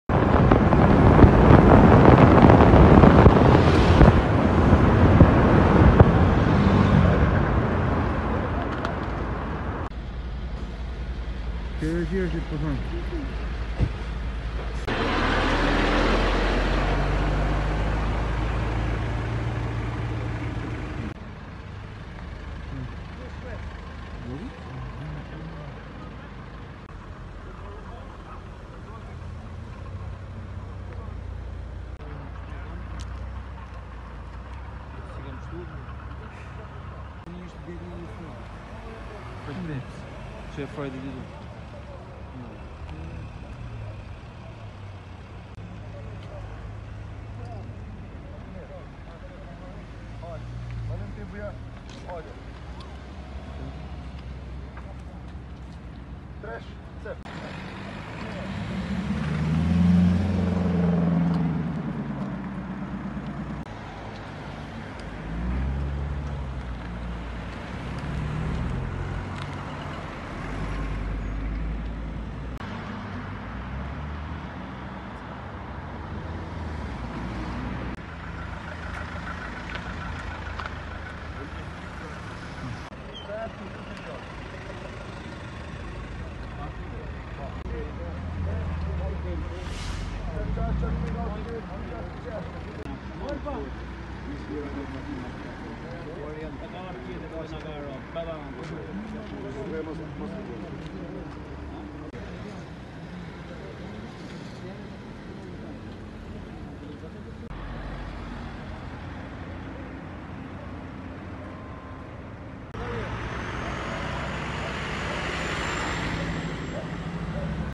Report TV - Përplasen 3 makina në superstradën Lezhë-Milot! Dy të plagosur nisen në spital
Një aksident ka ndodhur para pak minutash në superstradën Lezhë-Milot.

3 makina janë përplasur në afërsi të kryqëzimit të Rrilës. Paraprakisht raportohet për 2 persona të lënduar të cilët janë nisur me urgjencë në spitalin e Lezhës.

Policia ka shkuar në vendin e ngjarjes dhe po punon për zbardhjen e saj.

Si pasojë e aksidentit është krijuar trafik i rënduar përgjatë superstradës.